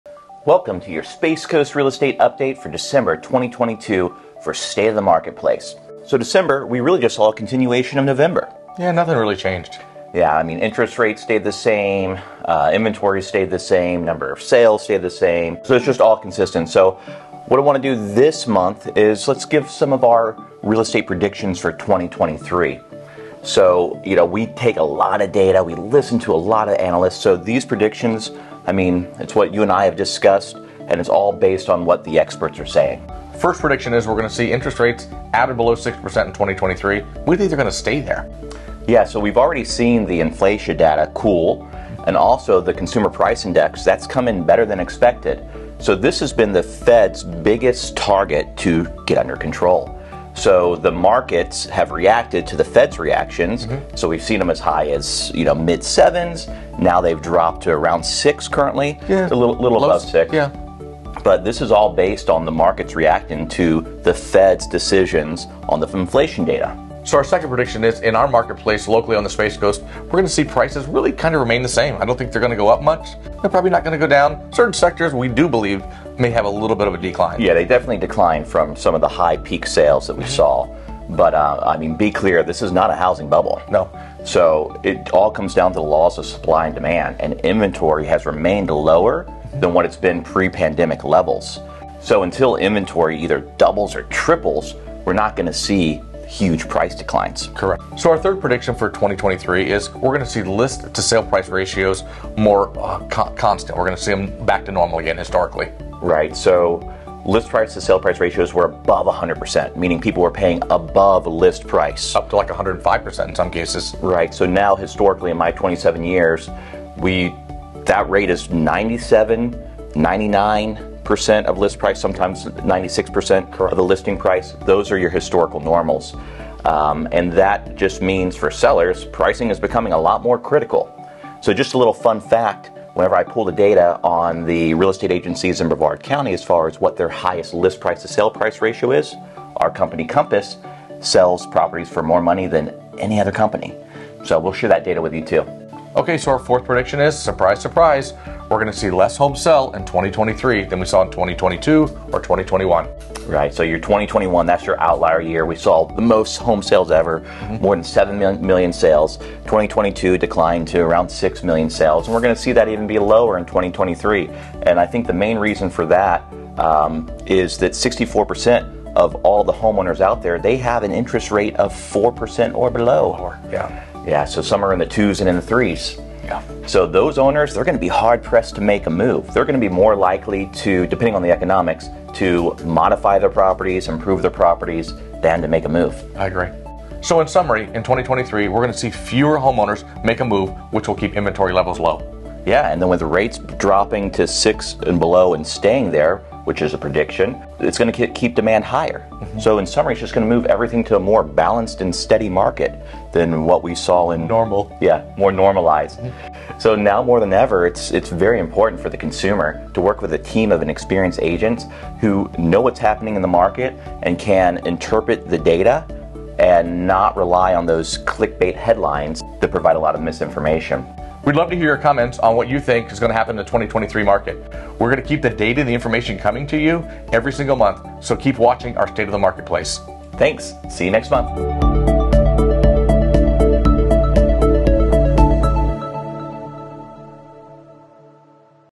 Real Estate Market Predictions for 2023 | Carpenter Kessel + Compass | SOTM
Welcome to our series "State of the Marketplace" - December 2022, Episode 20.

In our latest episode of SOTM, "Real Estate Market Predictions", Dewayne Carpenter & Kirk Kessel use their experience, research, and insights from market experts to give you a better understanding of what to expect from the real estate market in 2023.

00:00 - Intro
00:33 - Prediction #1: Interest rates will be at our below 6%
01:08 - Prediction #2: Brevard & the Space Coast home prices will remain the same
01:41 - Prediction #3: List-to-sale price ratios will normalize
02:28 - Prediction #4: Fewer homes will sell in 2023 than in 2022 or 2021

dewayne carpenter,kirk kessel, carpenter kessel, compass real estate, space coast realtor, brevard county realtor, waterfront realtor